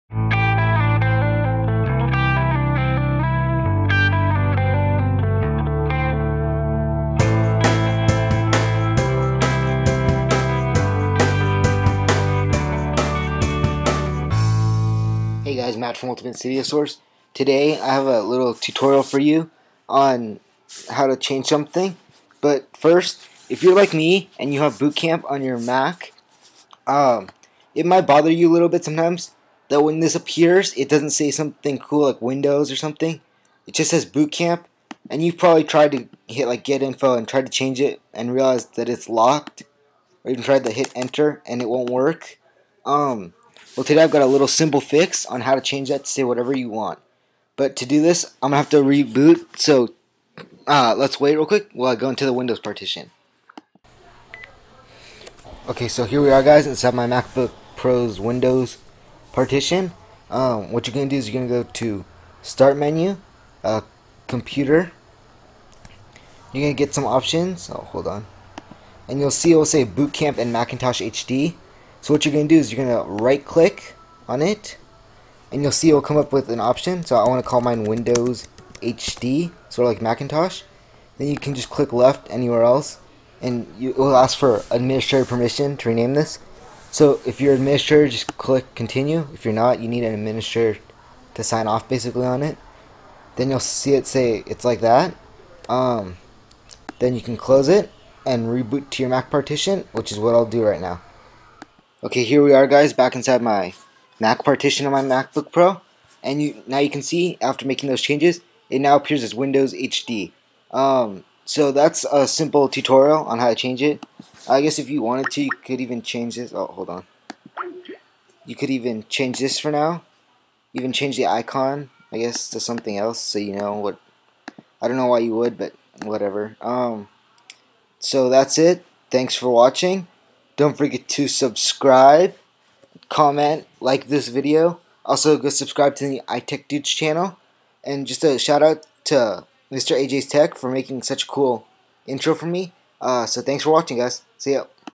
How to rename your Bootcamp Partition
This also works for regular windows pc's but it won't really make a difference.